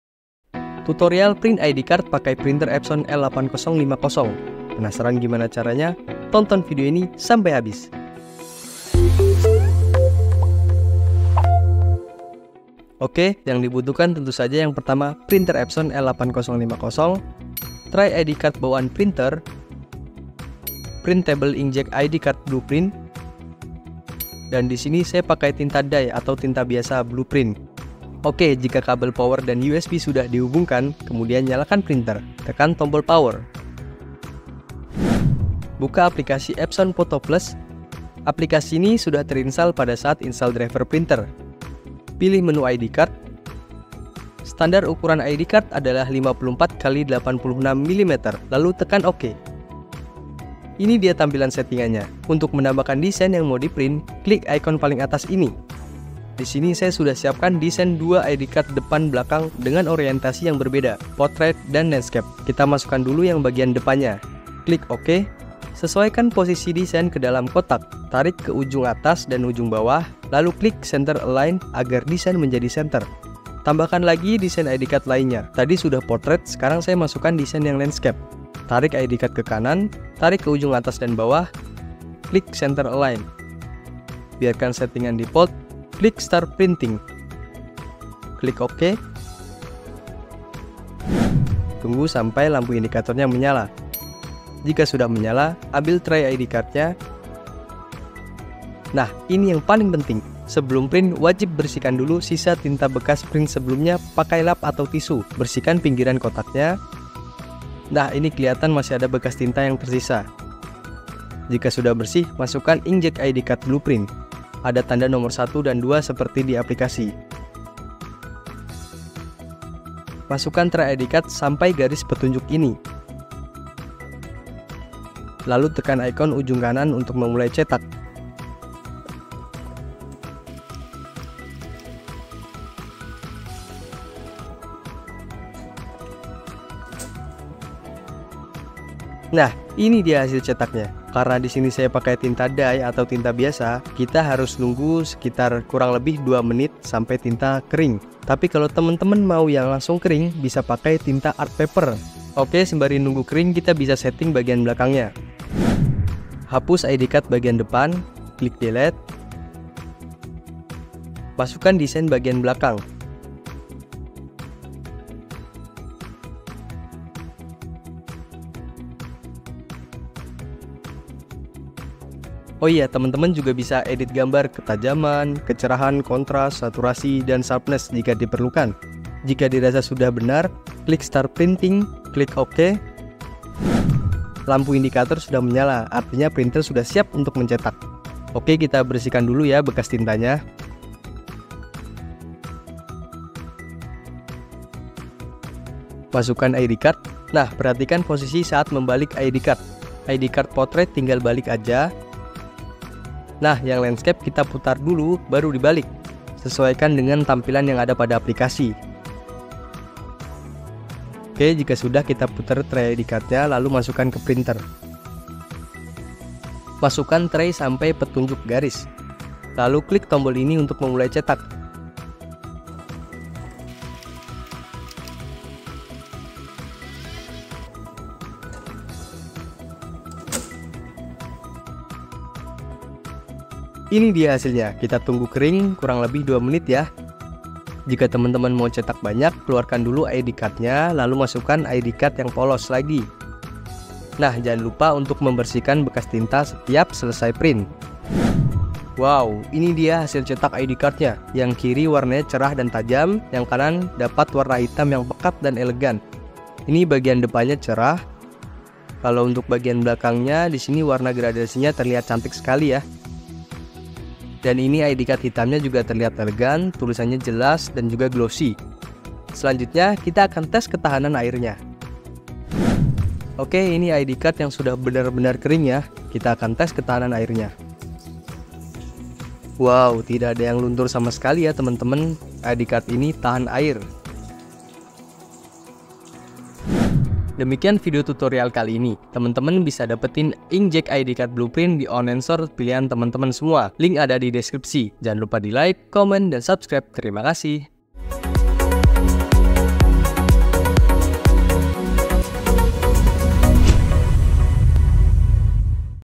Tutorial Print inkjet ID Card Blueprint pakai Printer Epson L8050 | BPVID#239
Pada video berikut tinta yang dipakai adalah tinta dye. jika mau cepat kering bisa pakai tinta art paper.

Tonton video sampai habis, jika ada pertanyaan boleh diketik di kolom komentar ya guys.